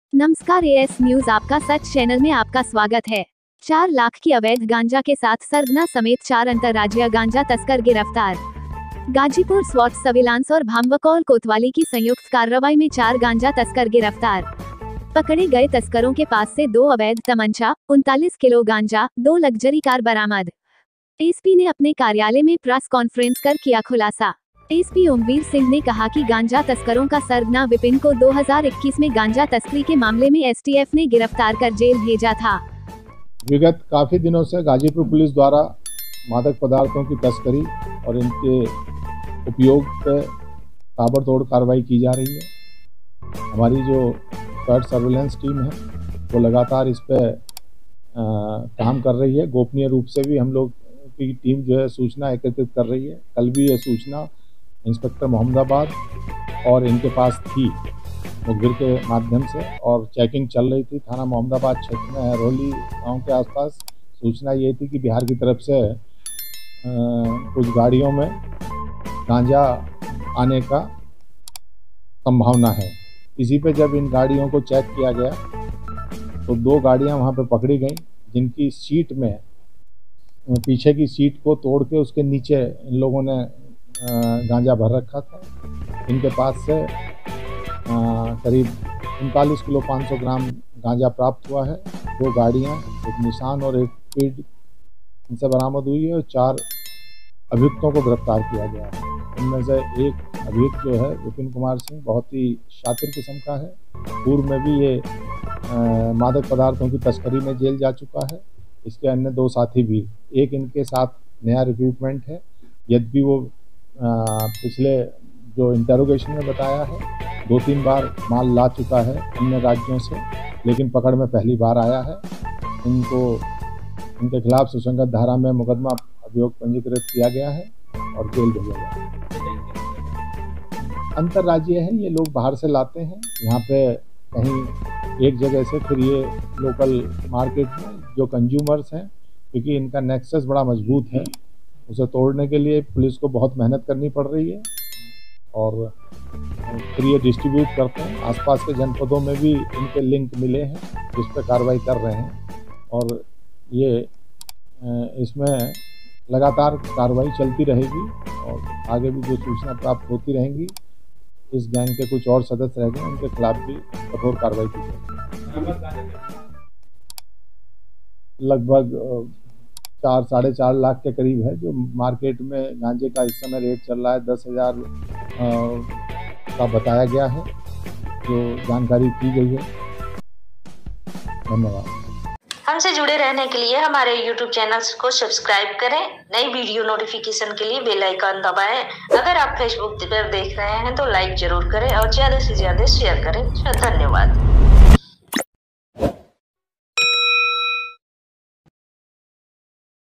Gjazipur Police ने 39 किलो से ज्यादा अवैध गांजा के साथ 4 तस्करों को गिरफ्तार किया, SP ने किया खुलासा
4 लाख की अवैध गांजा के साथ सरगना समेत 4 अन्तर्राजीय गांजा तस्कर गिरफ्तार

स्वाट सर्विलांस और भांवरकोल कोतवाली की संयुक्त कार्रवाई में 4 गांजा तस्कर गिरफ्तार

पकड़े गए तस्करों के पास से 2 अवैध तमंचा, 39 किलो गांजा, 2 लग्जरी कार बरामद

एसपी ने अपने कार्यालय में प्रेस कॉन्फ्रेंस कर किया खुलासा

एसपी ने कहा की गांजा तस्करों का सरगना विपिन को 2021 में गांजा तस्करी के मामले में एसटीएफ ने गिरफ्तार कर जेल भेजा था